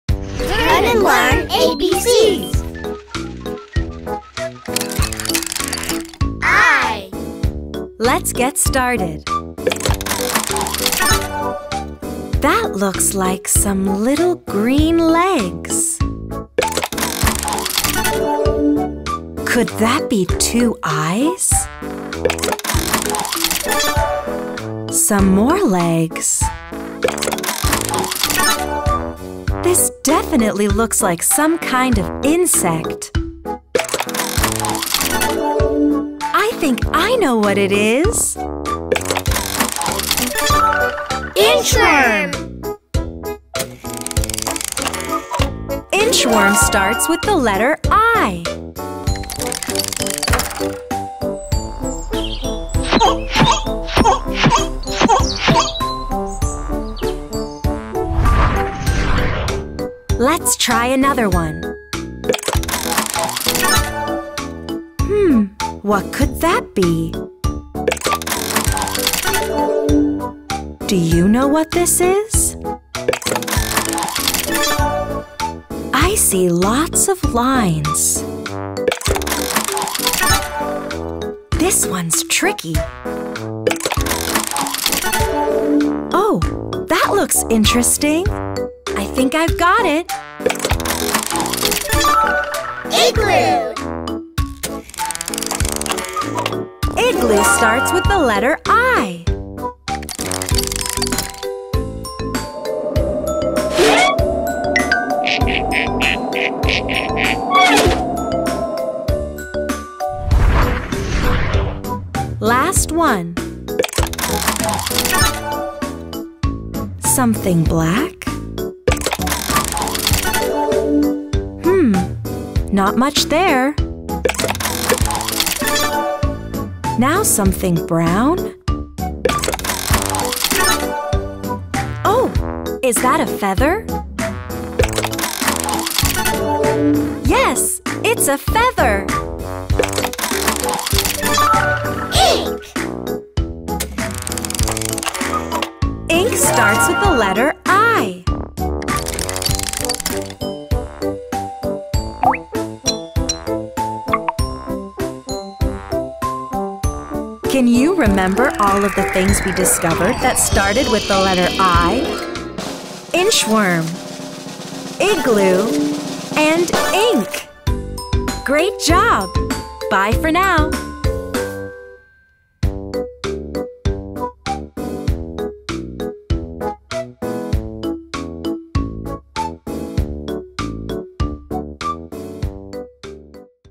Alphabet Surprise | Turn & Learn ABCs | Learn Letter | ​​🌈 Super Simple ABCs
Each week, learn a new letter of the alphabet with Turn & Learn ABCs, from the makers of Super Simple Songs. This week, learn letter I!